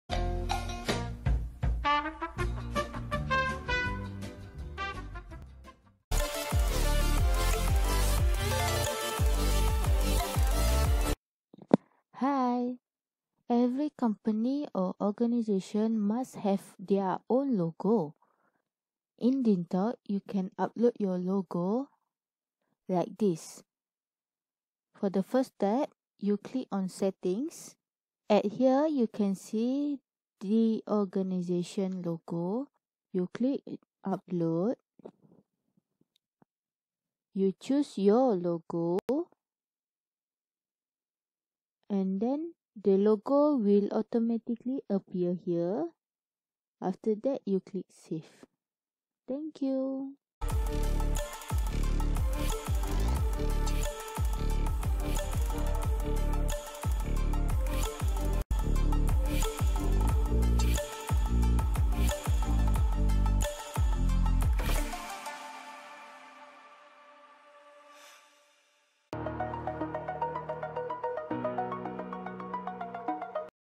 DINGTALK TOPIC 17: UPLOAD ORGANIZATION’S LOGO
This video will show you how to upload organization's logo in DingTalk application.